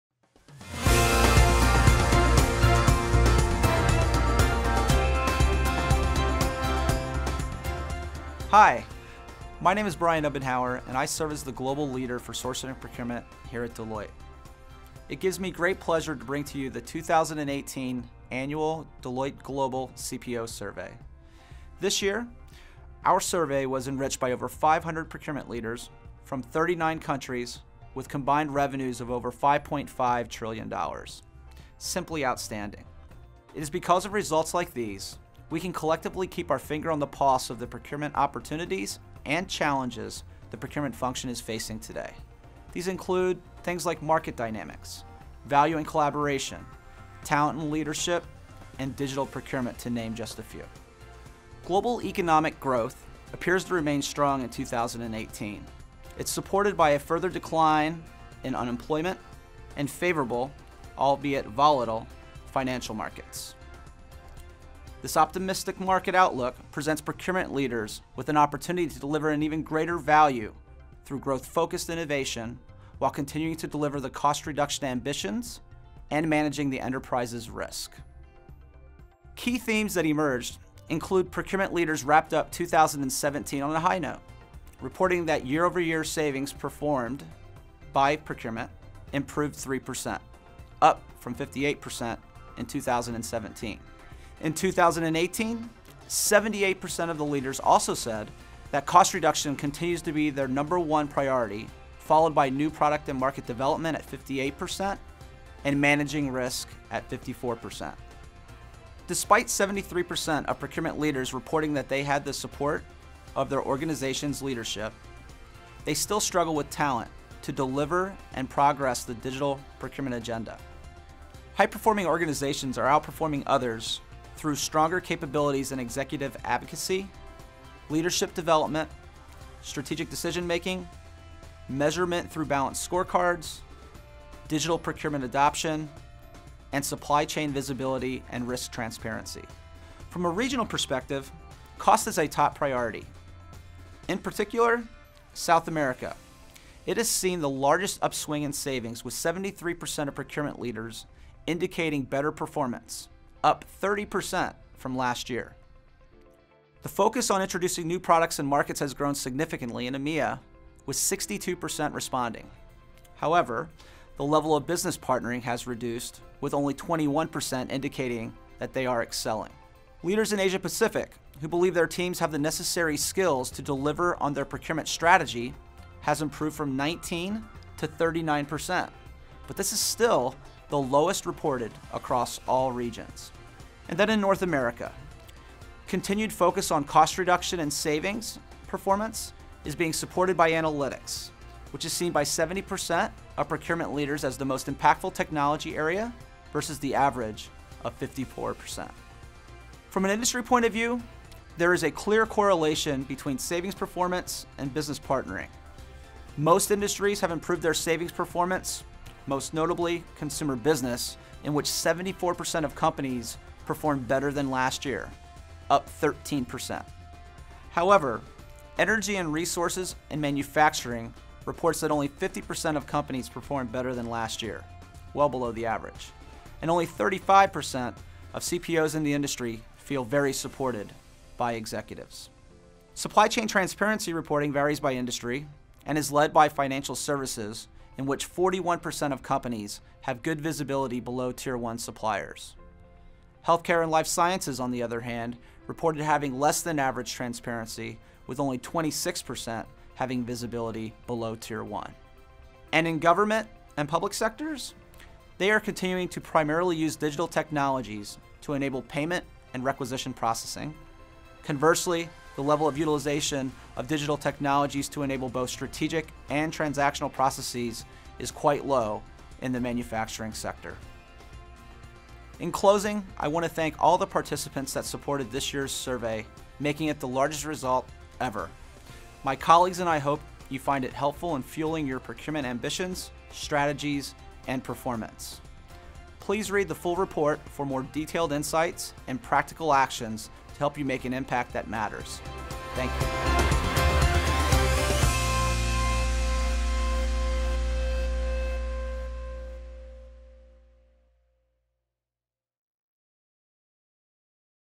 What are the key trends and issues impacting procurement leaders?
​The world economy enters 2018 with good momentum, presenting procurement leaders with an opportunity to deliver greater value through innovation while continuing to deliver cost reduction and managing risk. Watch Brian Umbenhauer, Principal and Global Head of Sourcing & Procurement with Deloitte Consulting LLP, share his views.